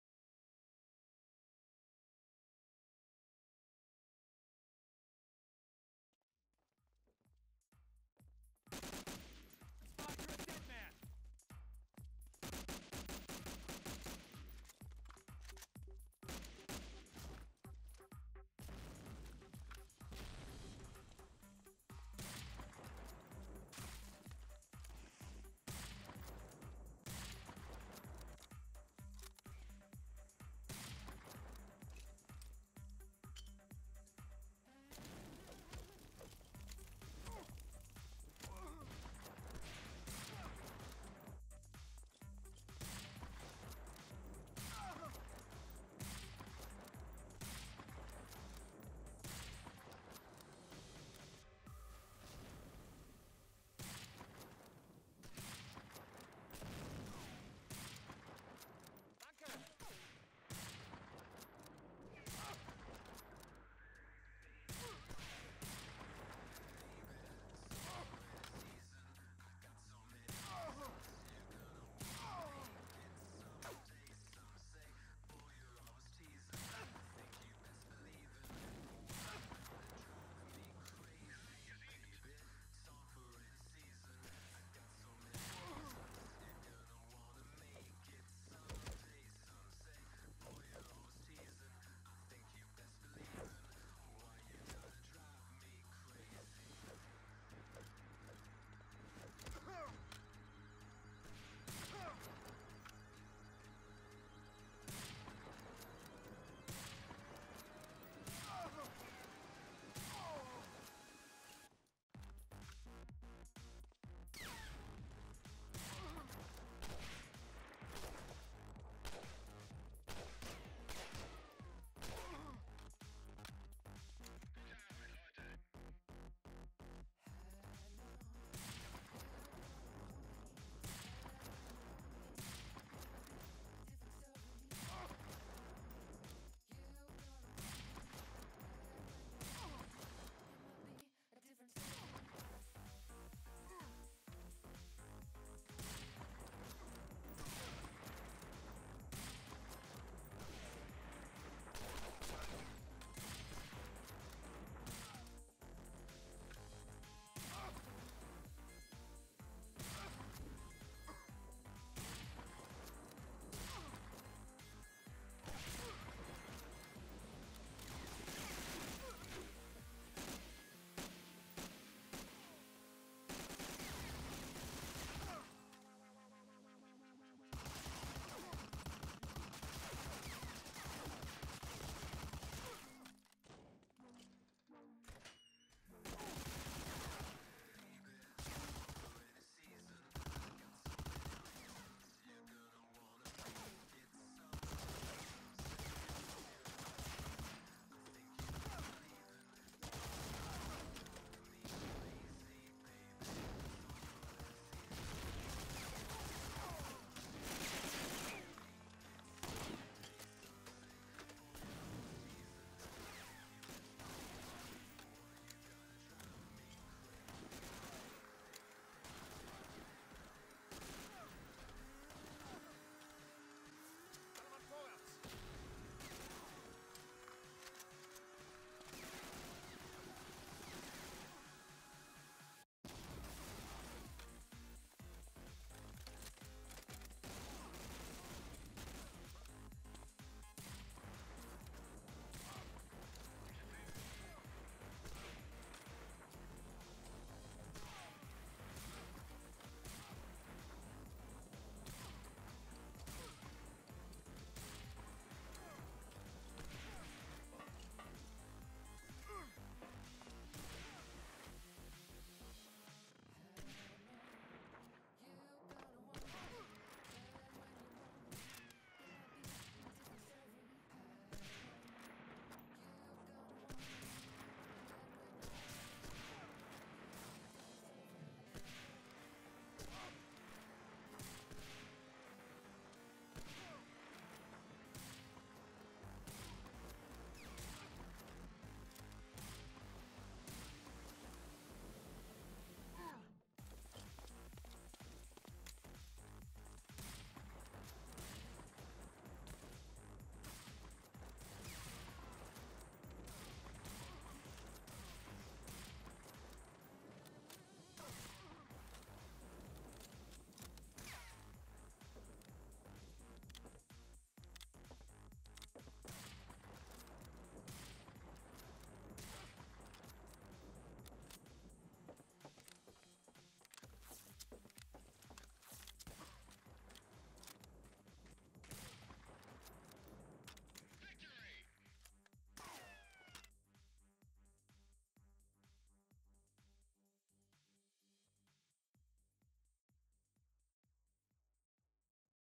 [FragMovie 2.0] Nasty Spirit^ [Final Version]
Some frags on a Public Server,
& some Wars on ScrimBase League.

I reuploaded it again for quality fix, But Sony Vegas Pro dont want 1080p...(Resolution issue)